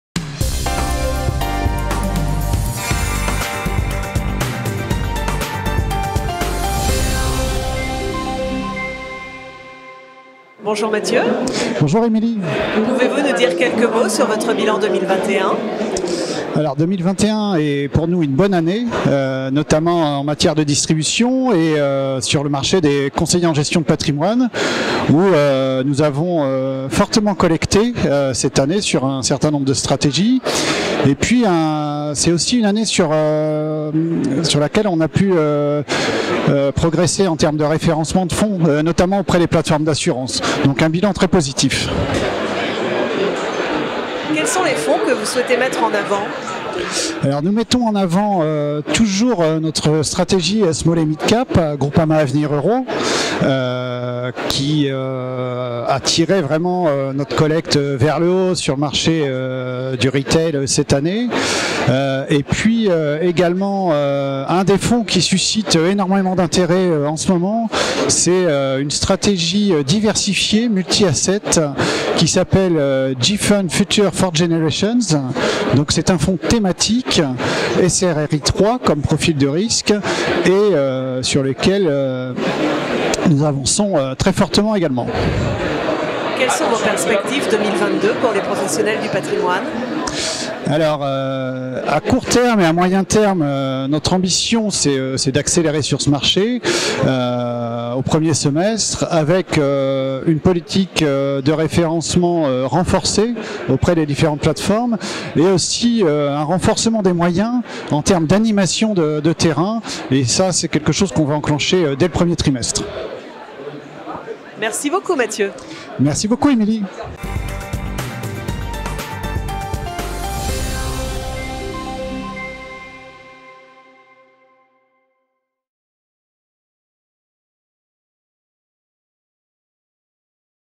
Sommet BFM patrimoine/CNCGP – Groupama AM
Retrouvez Matthieu PAINTURAUD - Directeur de la distribution - Groupama AM